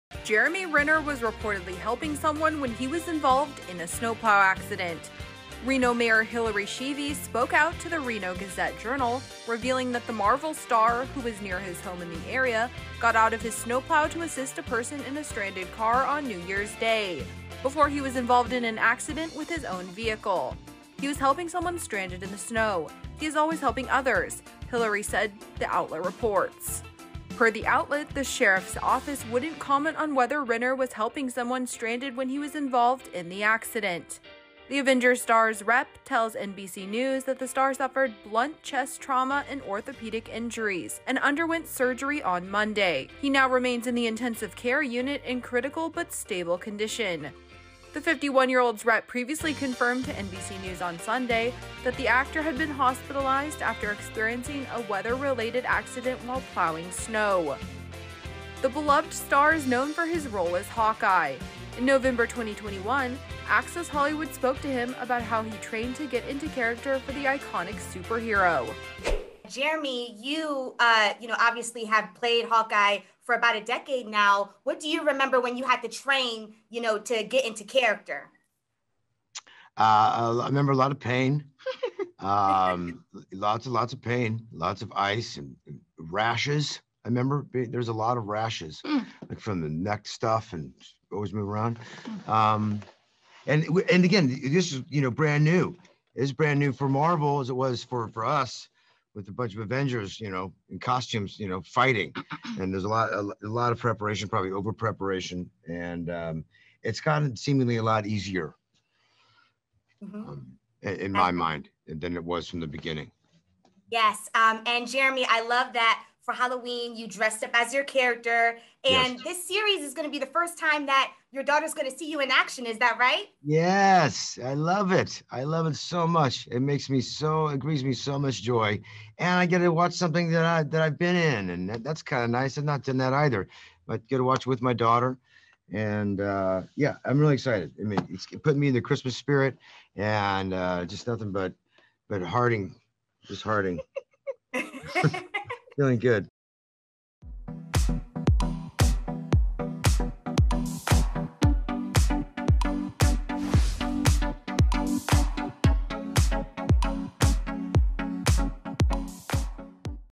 Jeremy Renner Was ‘Helping Someone Stranded’ Before Snowplow Accident (Report)
Jeremy Renner was reportedly helping someone when he was involved in a snowplow accident. Reno Mayor Hillary Schieve spoke out to the Reno Gazette Journal, revealing that the Marvel star got out of his snowplow to assist a person in a stranded car before he was involved in an accident with his own vehicle.

Jeremy Renner Was ‘Helping Someone Stranded’ Before His Snowplow Accident (Report)